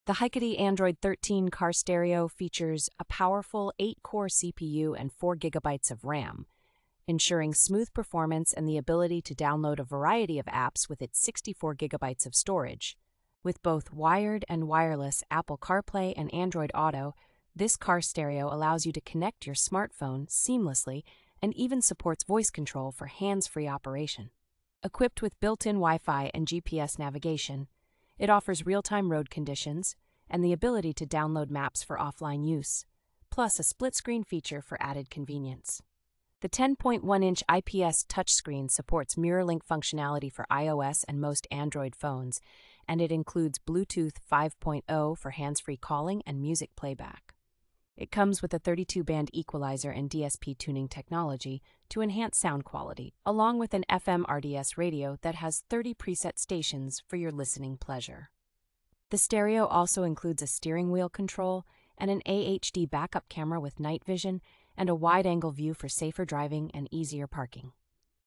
REVIEW (2024): Hikity Android 13 Car Stereo. ESSENTIAL details.
The Hikity Android 13 Car Stereo features a powerful 8-core CPU and 4 gigabytes of RAM, ensuring smooth performance and the ability to download a variety of apps with its 64 gigabytes of storage.

With both wired and wireless Apple Carplay and Android Auto, this car stereo allows you to connect your smartphone seamlessly and even supports voice control for hands-free operation.

Equipped with built-in WiFi and GPS navigation, it offers real-time road conditions and the ability to download maps for offline use, plus a split-screen feature for added convenience.

The 10.1-inch IPS touchscreen supports mirror link functionality for iOS and most Android phones, and it includes Bluetooth 5.0 for hands-free calling and music playback.

It comes with a 32-band equalizer and DSP tuning technology to enhance sound quality, along with an FM/RDS radio that has thirty preset stations for your listening pleasure.

The stereo also includes a steering wheel control and an AHD backup camera with night vision and a wide-angle view for safer driving and easier parking.